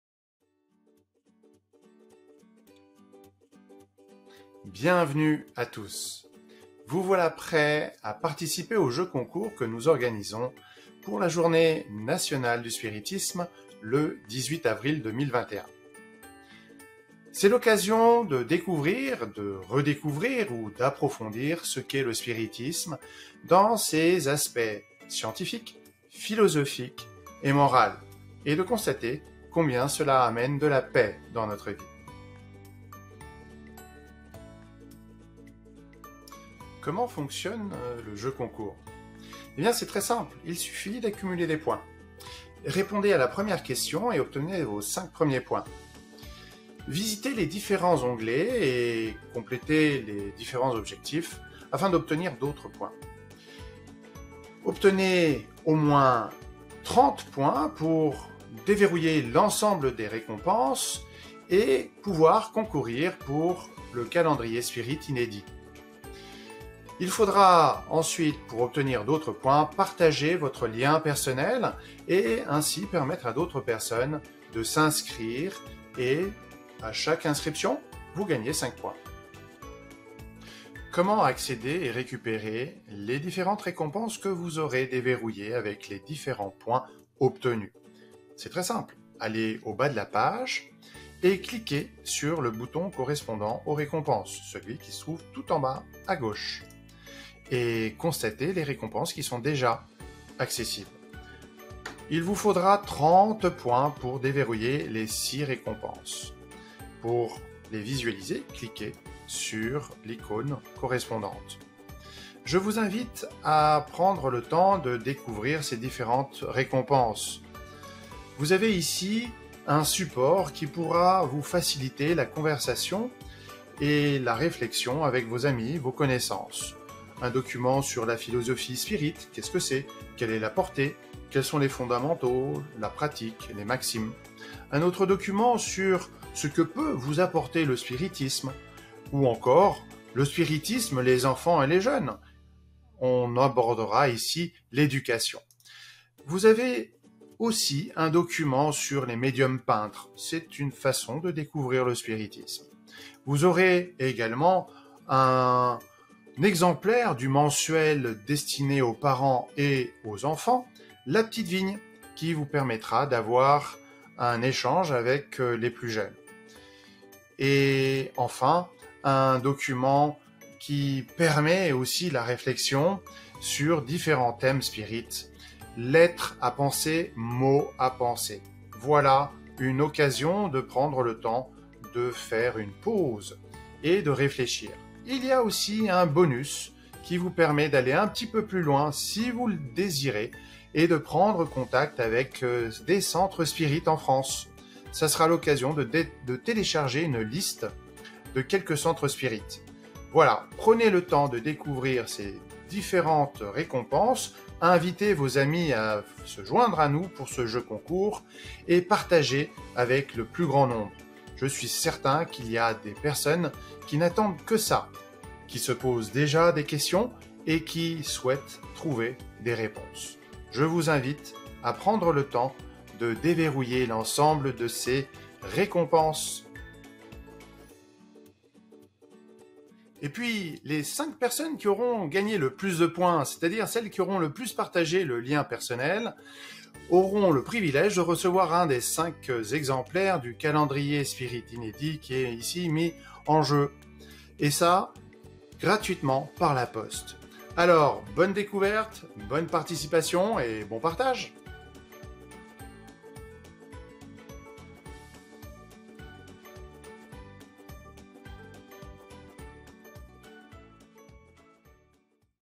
JNS 2021 jeu Concours - LA MAISON DU SPIRITISME
Le 18 avril 2021, Journée Nationale du spiritisme, est une occasion
pour chaque spirite de partager avec tous ceux
qui cherchent à comprendre la vie spirituelle,
l'apaisement que le spiritisme leur apporte.

Nous vous invitons à participer au Jeu-Concours
que nous organisons pendant un mois
du 18 mars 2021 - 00H00 au 18 avril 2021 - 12H00.

6 récompenses par participant et 5 Grand-Prix en tout sont en jeu
Tous les participants gagnent des récompenses immédiatement !
Et les 5 personnes qui auront le plus partagé ce Jeu-Concours
gagneront le Grand-Prix à la clôture du concours:

Un CALENDRIER SPIRITE 2020/2021 inédit
avec une illustration et un principe spirite à méditer chaque mois
pour mieux comprendre la vie spirituelle.

Une fois inscrit, vous pourrez récupérer votre lien personnel
qui vous permettra de cumuler les points
et de remporter le Grand-Prix !!!

Joyeux partages et Bonne Chance !

Fraternellement,

Les équipes du CESVAK et de LA MAISON DU SPIRITISME.